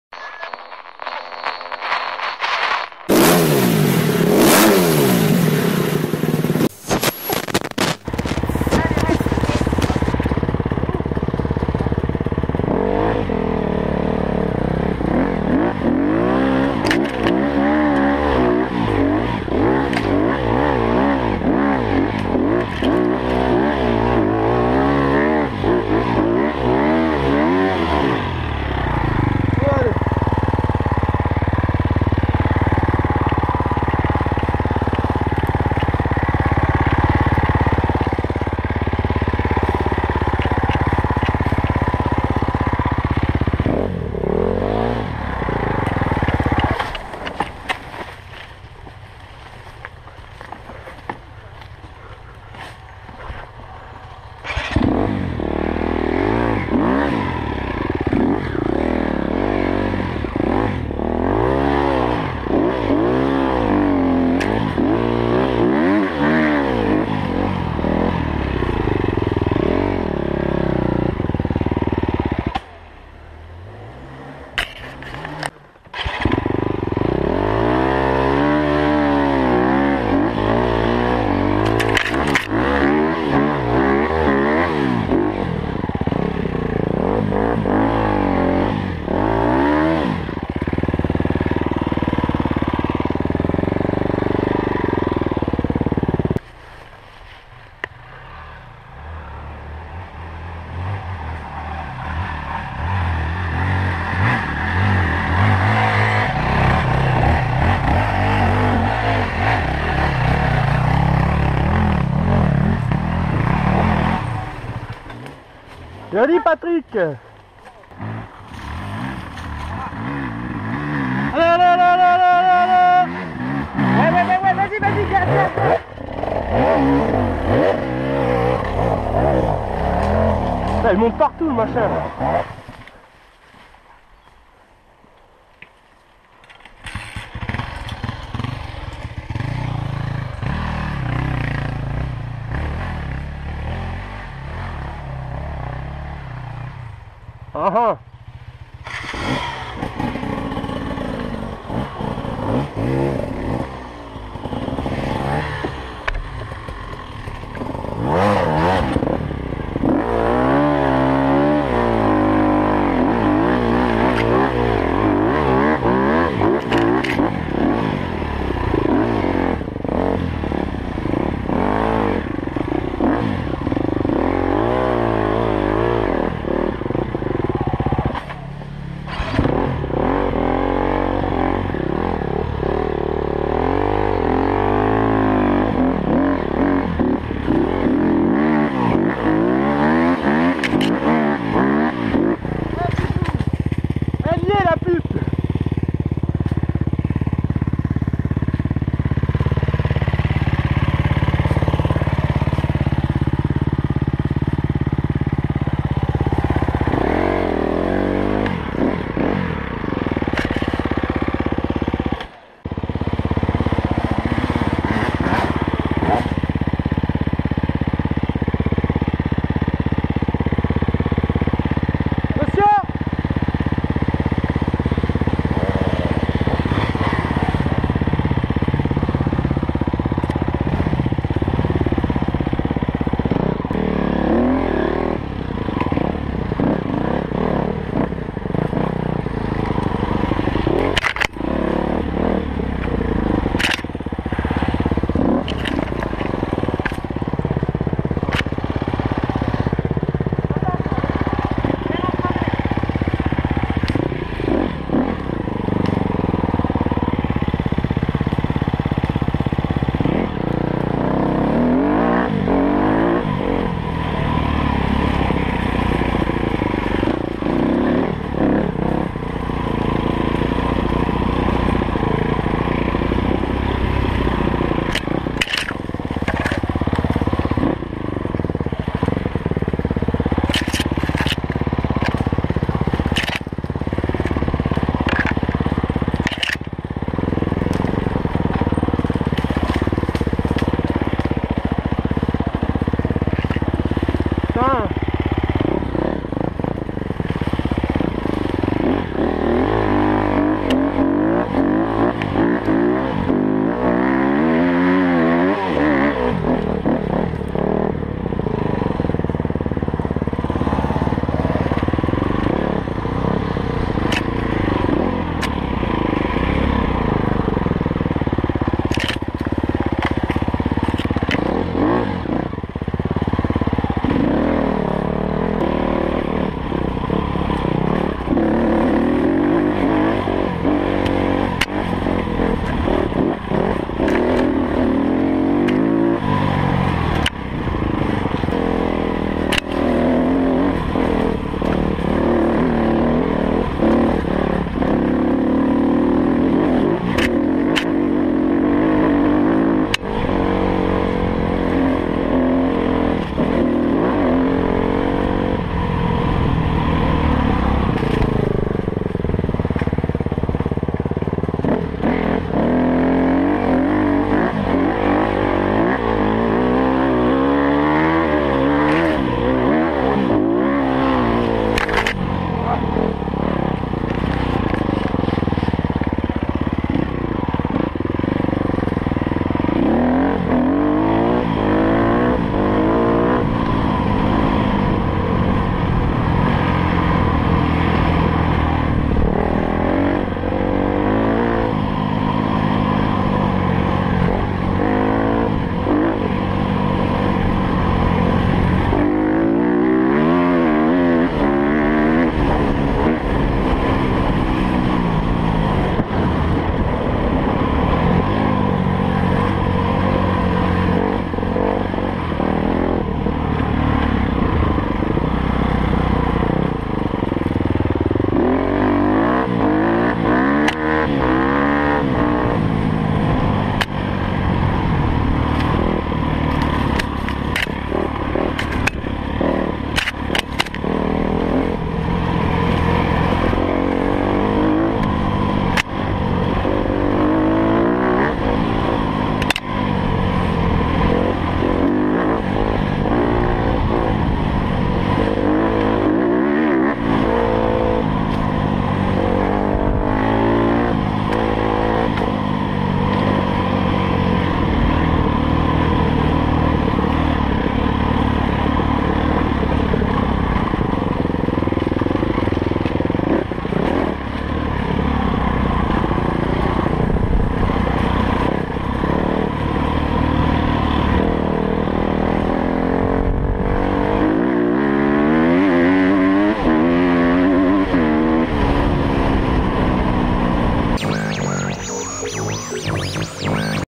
Sortie 11.1 Enduro Bourgogne 250 TE/300 EXC/450 WRF GoPro Hero 3 Black Free ride Franchissement!!!!!
Salut a tous amis riders la saisons reprend et les vidéos aussi :)
Voila la suite de la sortie 11! :)
Si vous êtes bourguignon venez faire un tour sur ma page Facebook: